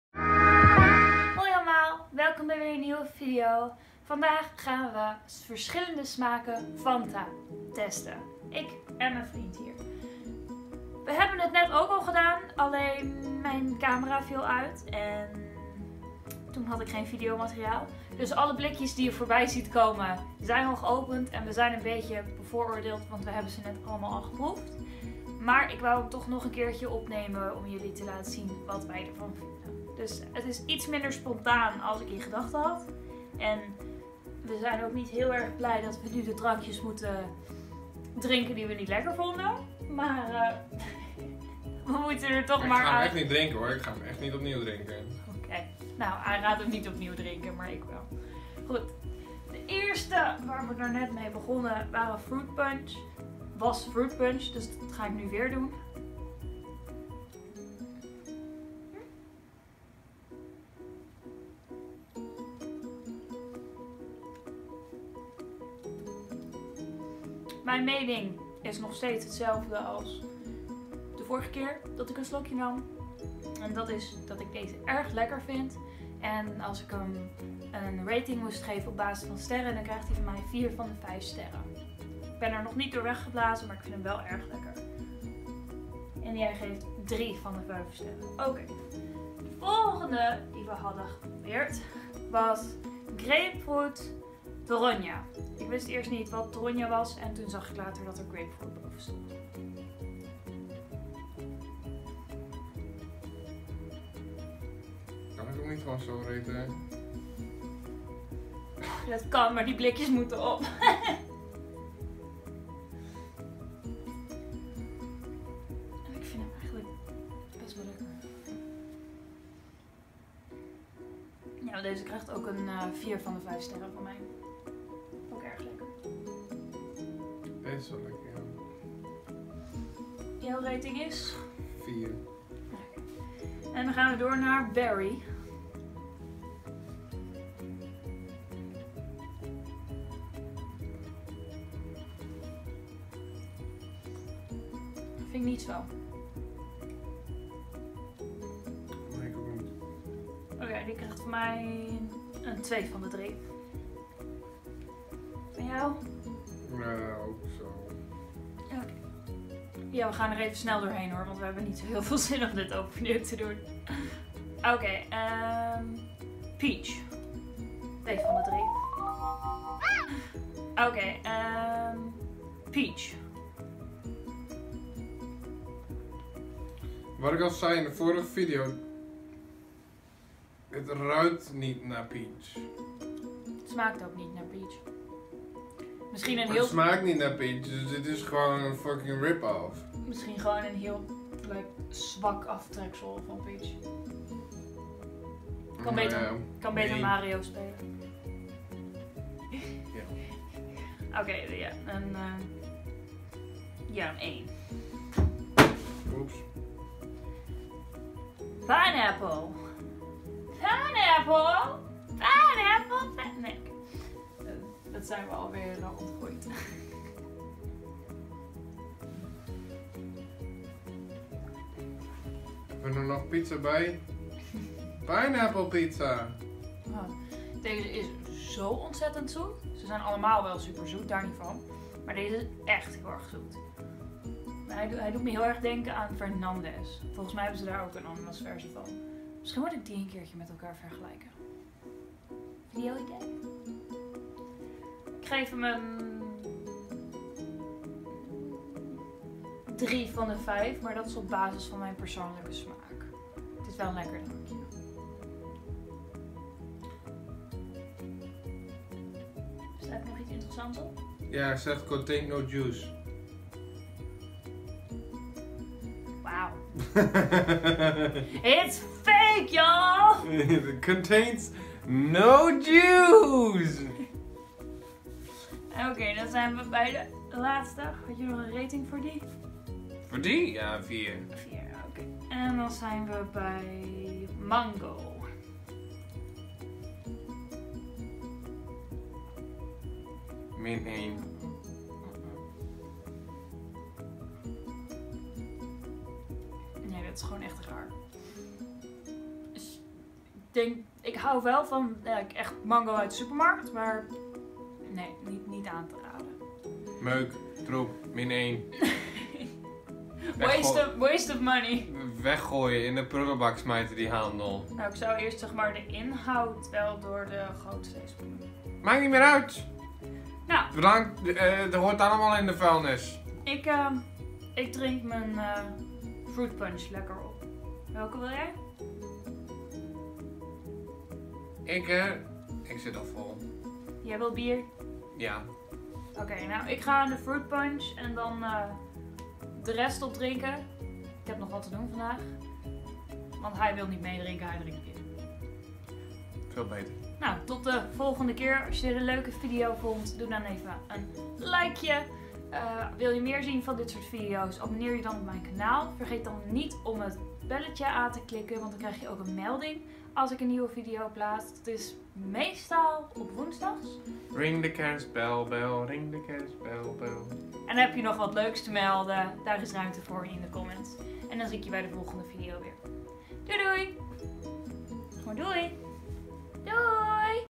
#22 Fanta proeven ft Masterflotse
Samen met mijn vriend Hotzeharm aka Masterflotse heb ik wat gekke fanta smaken geproefd. De blikjes komen van de big bazaar.

Veel kijkplezier.